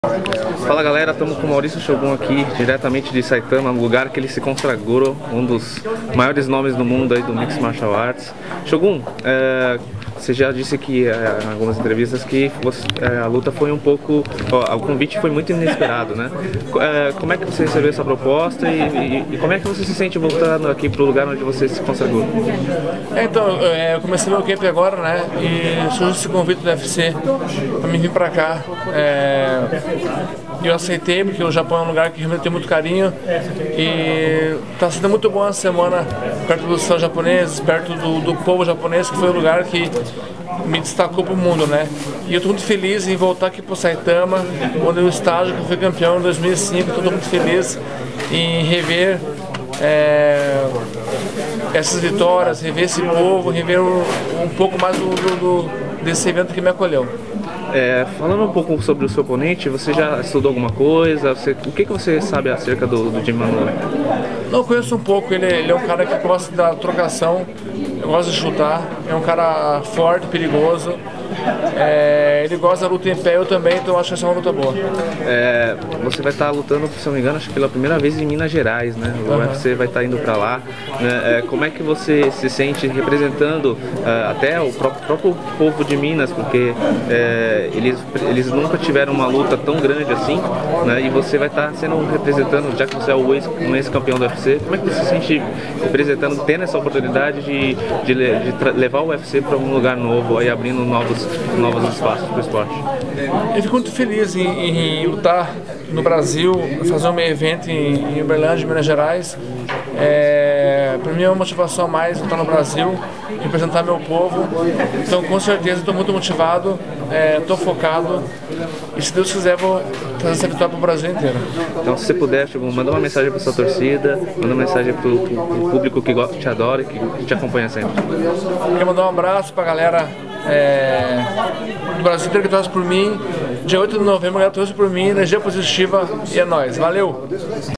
Maurício Shogun fala sobre Jimi Manuwa e UFC Uberlândia - Gazeta Esportiva.net
Planeta Octógono - Gazeta Esportiva.net